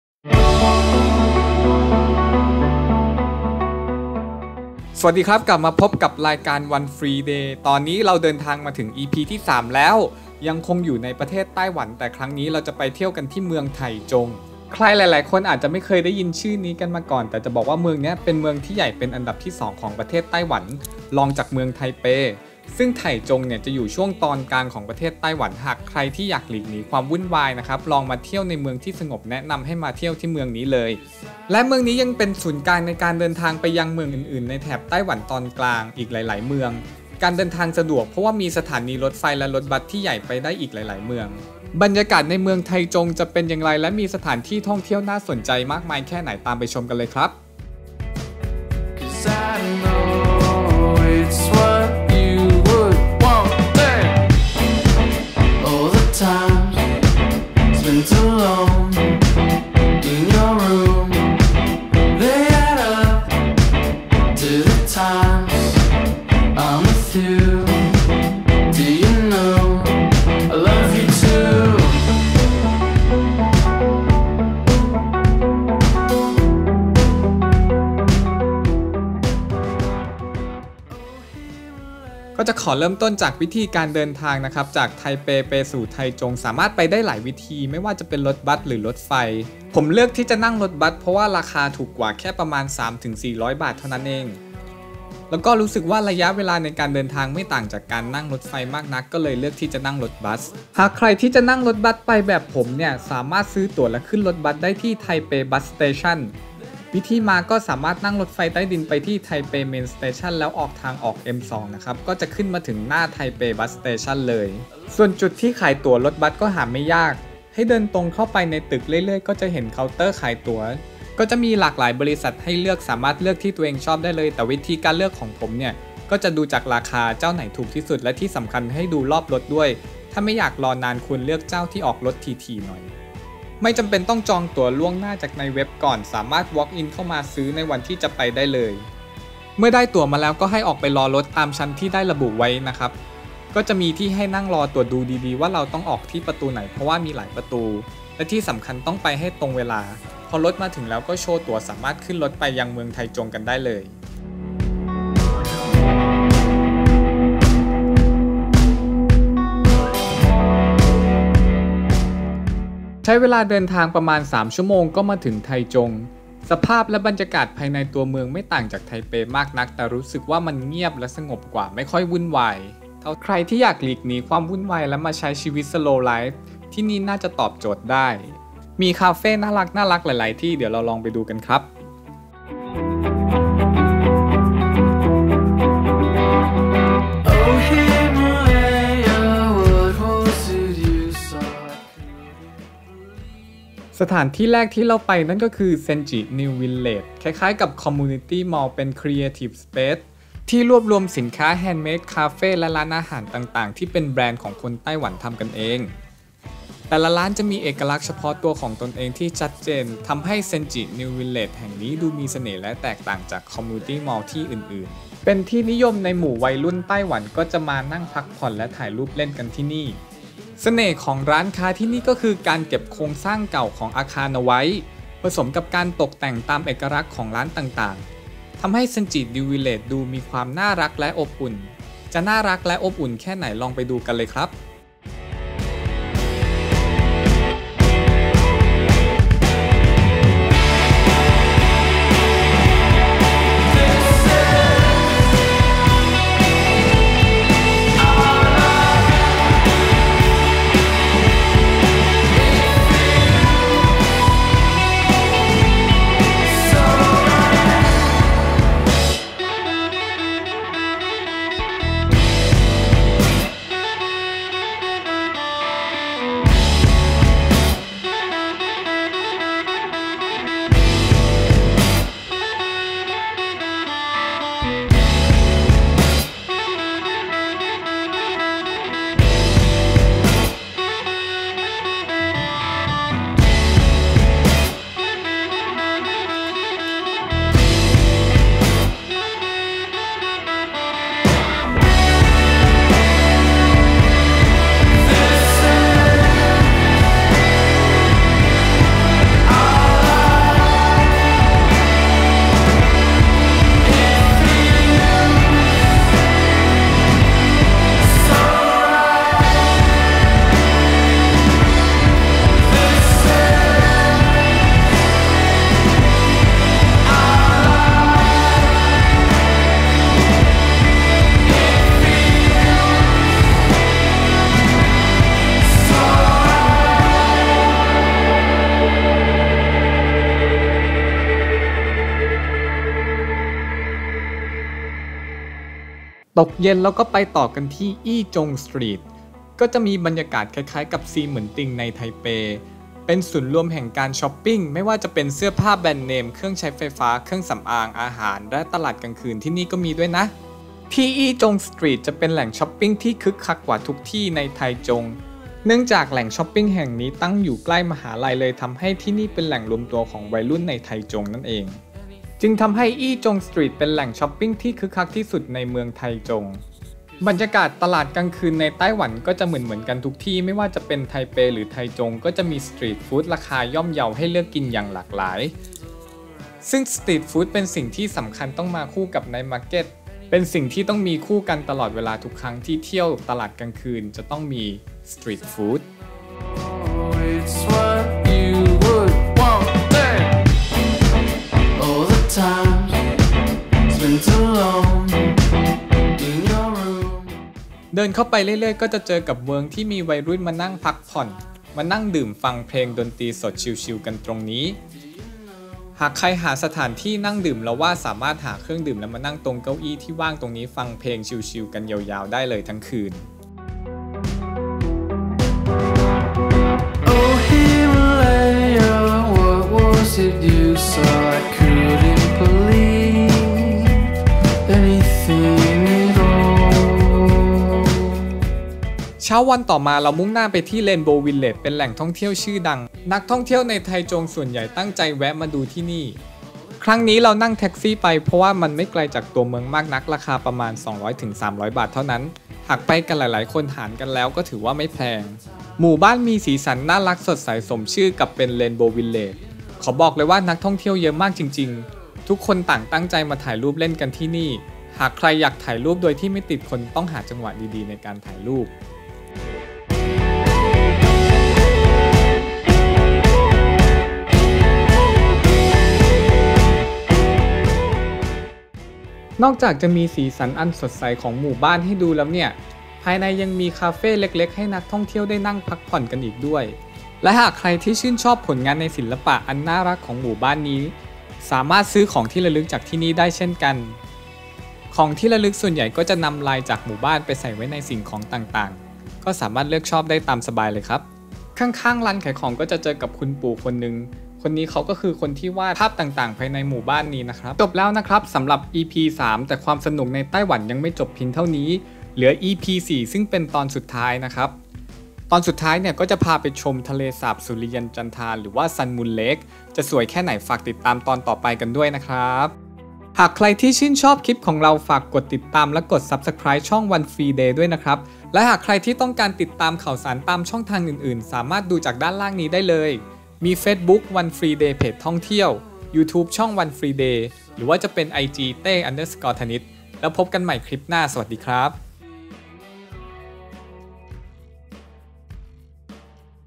เที่ยวไต้หวัน Taiwan EP.3 : เที่ยวไถจง (Taichung) [One free day]
พาเที่ยวเมือง Taichung (ไถจง) ใครหลายคนอาจจะไม่เคยได้ยินซื่อนี้มาก่อน แต่จะบอกว่าเมืองนี้เป็นเมืองที่ใหญ่เป็นอันดับที่ 2 ของประเทศไต้หวันรองจากเมืองไทเป ซึ่งไถจงตั้งอยู่ในช่วงตอนกลางของประเทศไต้หวัน หากใครที่อยากหลีกหนีความวุ่นวายมาลองเที่ยวในเมืองที่สงบแนะนำให้มาที่เมืองนี้เลย และเมืองนี้ยังเป็นศูนย์กลางในการเดินทางออกไปเที่ยวเมืองต่างๆในแถบไต้หวันตอนกลางได้อีกด้วย การเดินทางสะดวกมีสถานีใหญ่ที่รวมทั้งรถไฟและรถบัสไว้ในบริเวณใกล้เคียงกัน จะมีอะไรน่าสนใจบ้างไปดูกันเลยครับ

หากใครที่ต้องการจะอ่านเป็นบทความ
สามารถดูได้ที่เพจ One free day ตามลิ้งด้านล่างนี้เลยครับ